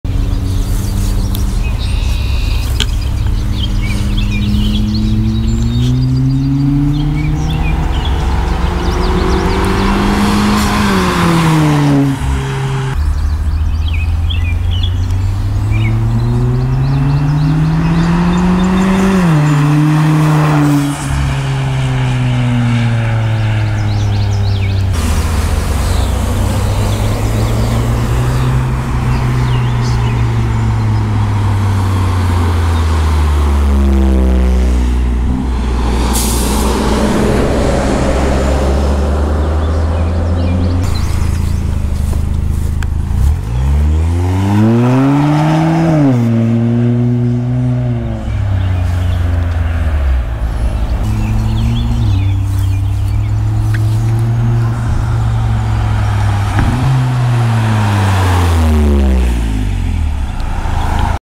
Corksport 80mm Catback
A short flyby clip with the Corksport 80mm Catback on a Mazda 3 Turbo Hatchack (M3TH). The Catback has been on the M3TH for more than a year now.